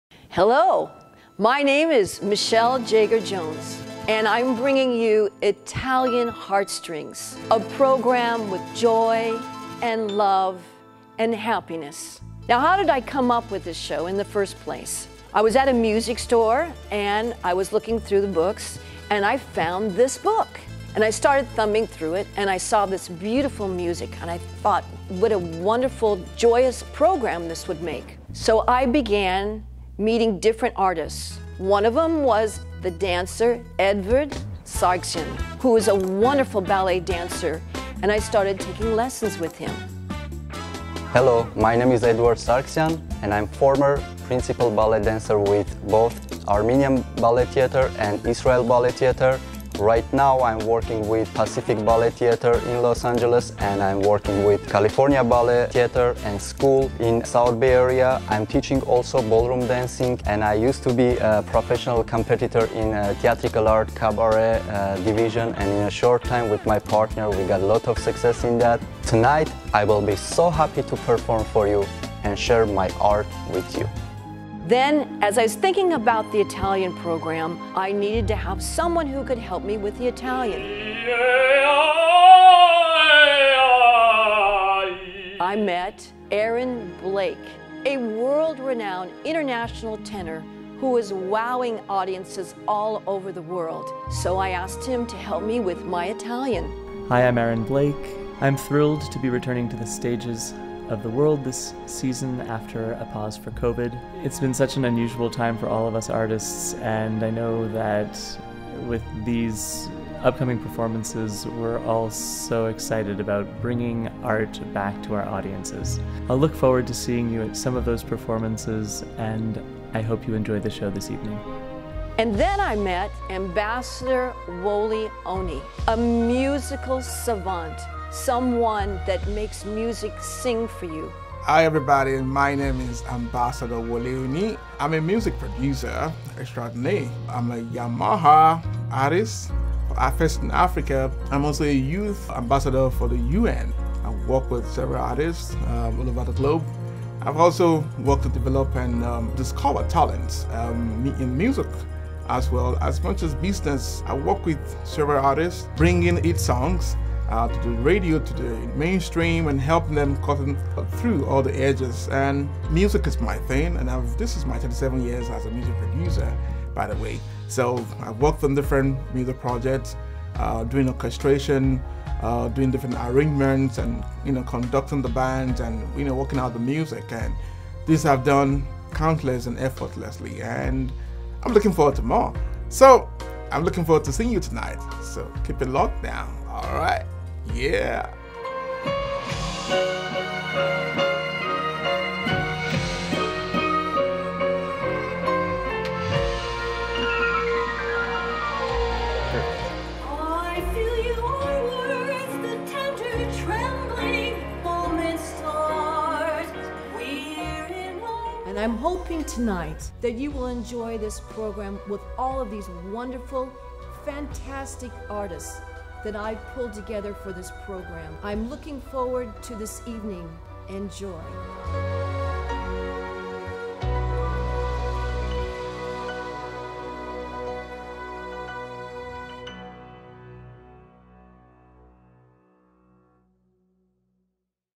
Italian Heart Strings show promotion
Promotional advertisement for Italian Heart Strings - the stage show .  This promotional video will be used as the introduction to the show when it is held at a theater or showcase.  This video introduces: Aaron Blake, Tenor, Eduard Sargsyan, Premier Dancer, and Ambassador Wole Oni, Musical Director.